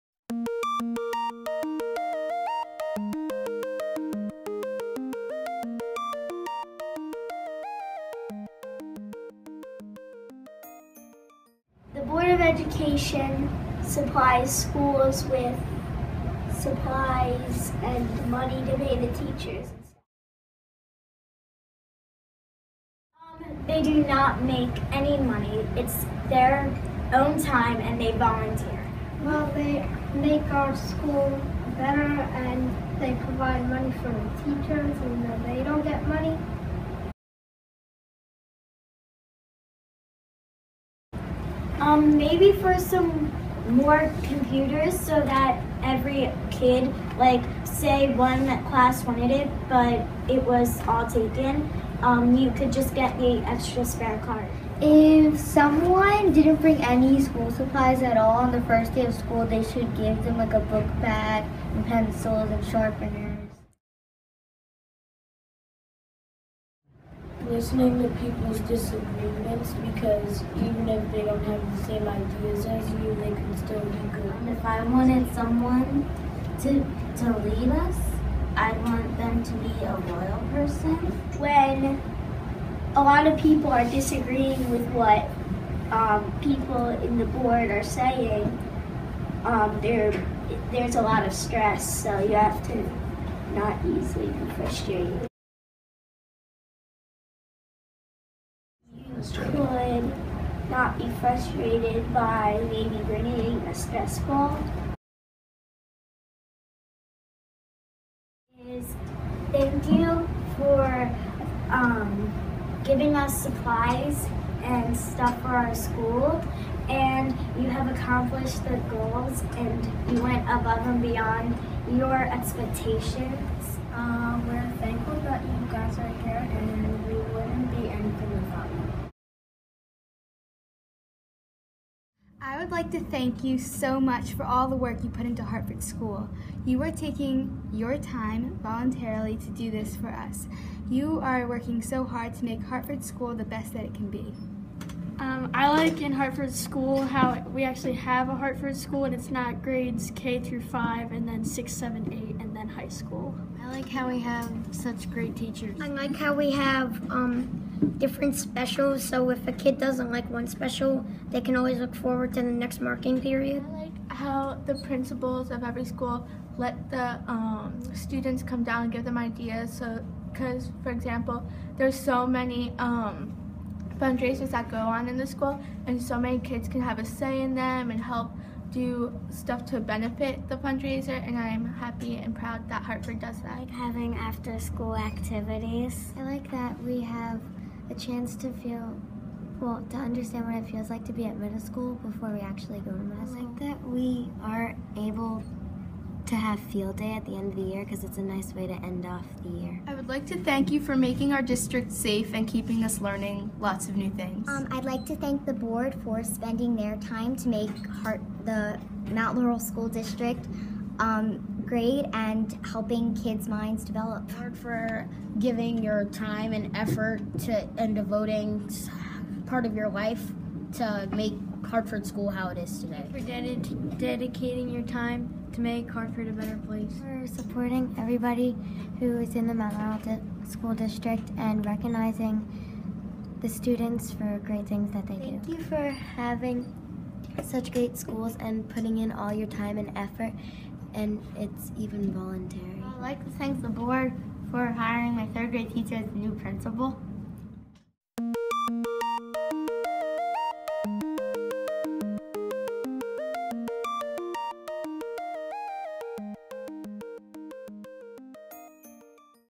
BOE Recognition, 2016
January is Board of Education Recognition Month in New Jersey. This year, students at Parkway and Hartford Schools shared their thoughts on the work of our board.